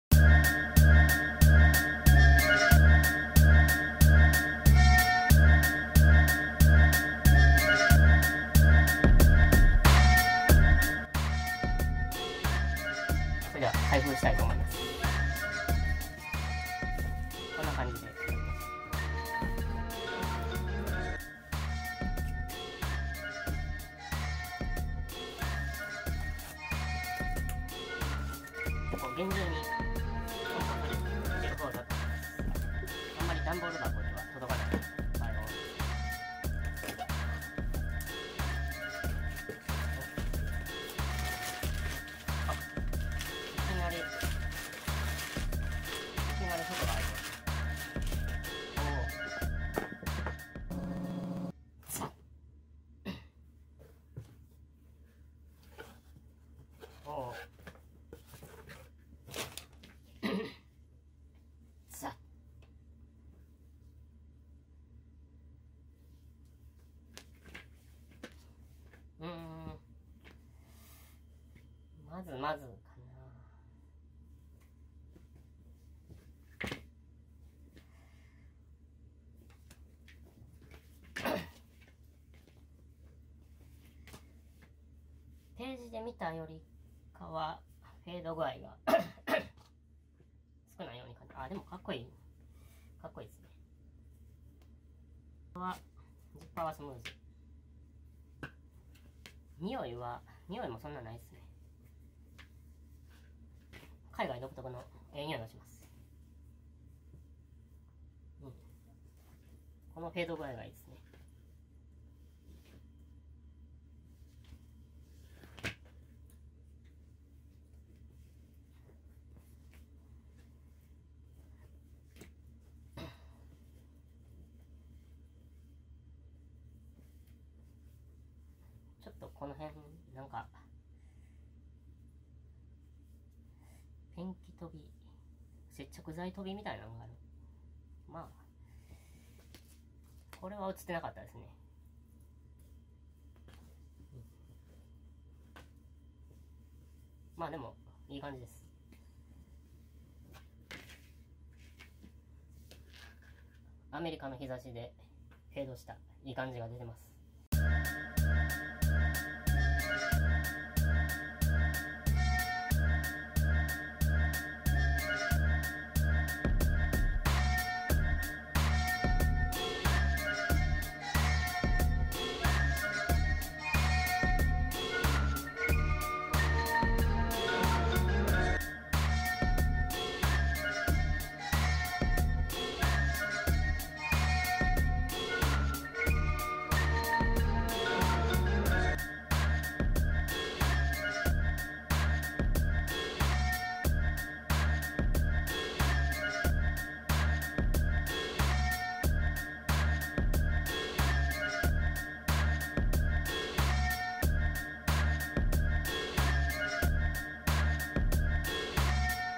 【carhartt】それこそ本場アメリカでSunfadeした古着が欲しい！今の気分はジップ式パーカー。しょ？
※古着動画ですので、完璧を求める方や細部にこだわる神経質な方は視聴をお控え下さい。